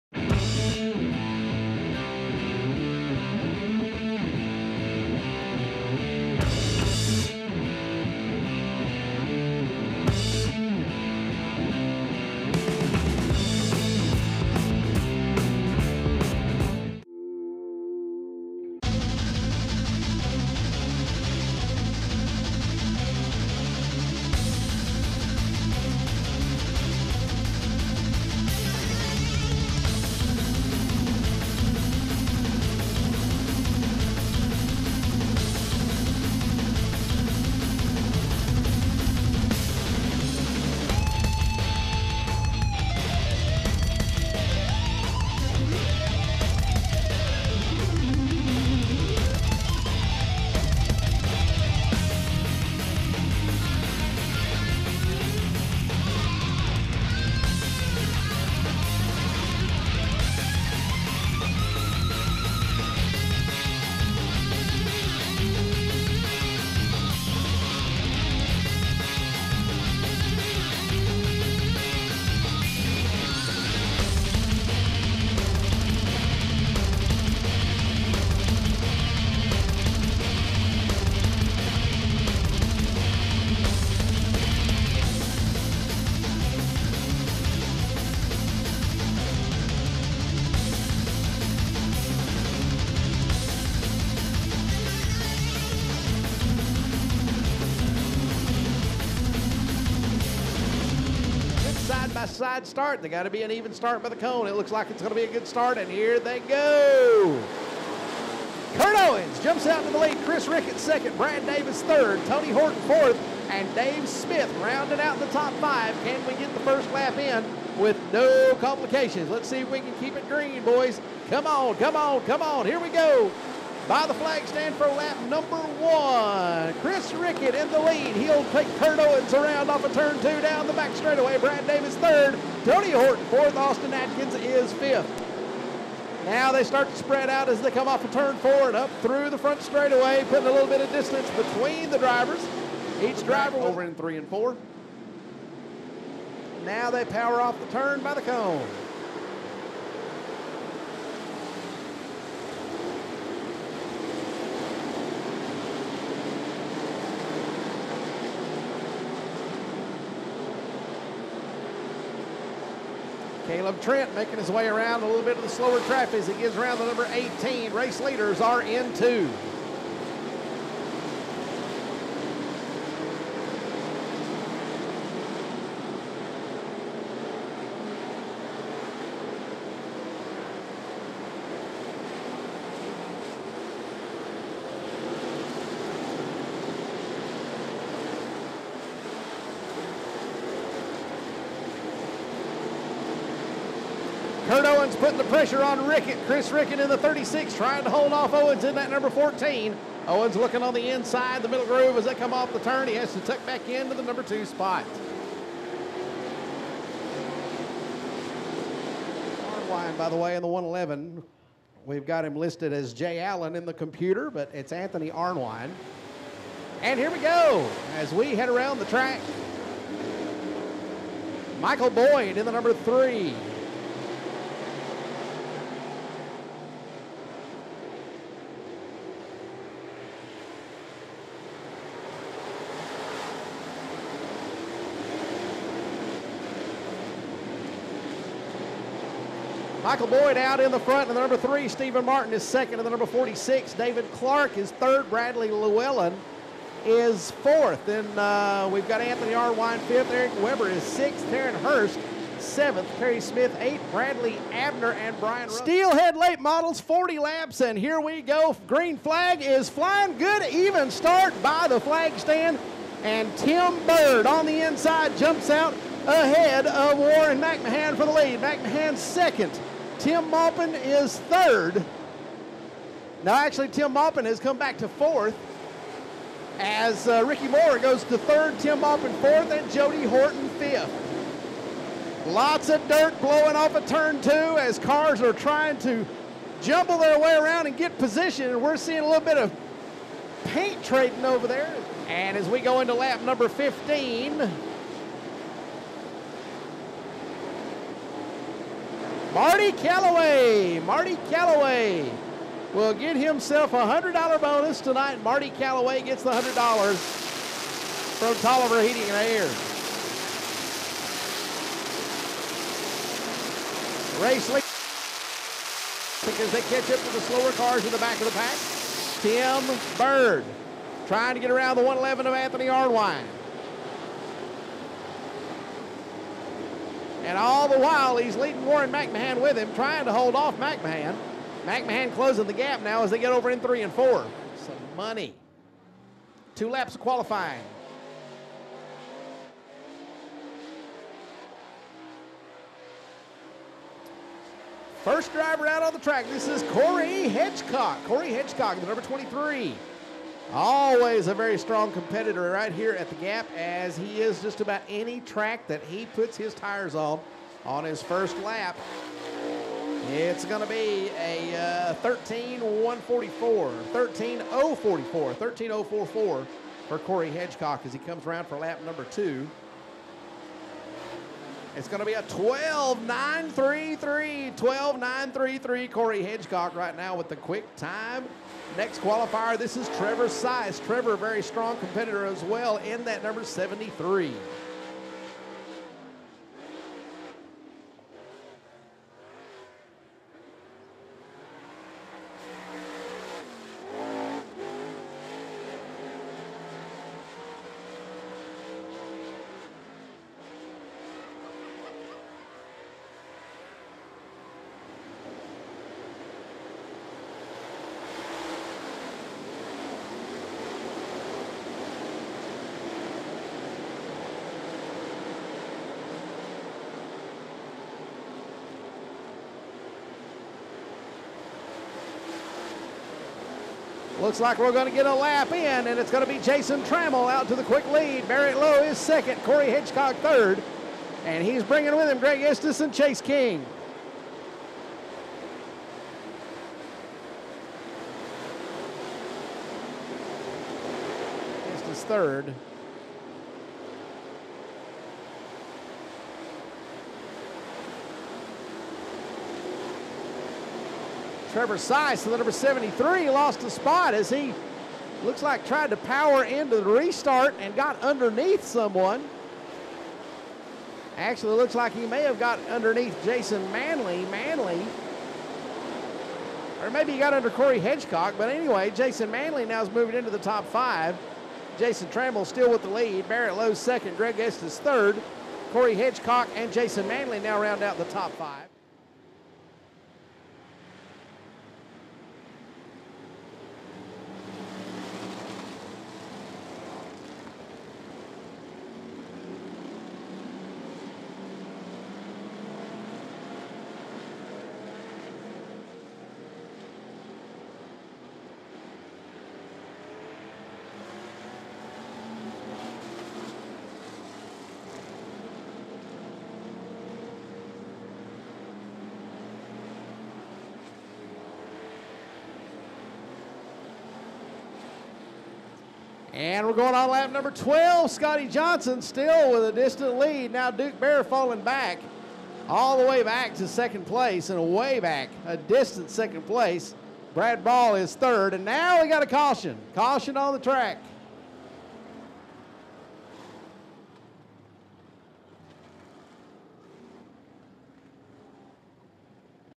Volunteer Speedway Highlight July 11 , 2015
Fan Appreciation Night at Volunteer Speedway
Steel Head 40 Laps for $2,000
Sportsman 25 Laps $700
Modified Hobby 25 Laps $500
Pure Stock 20 Laps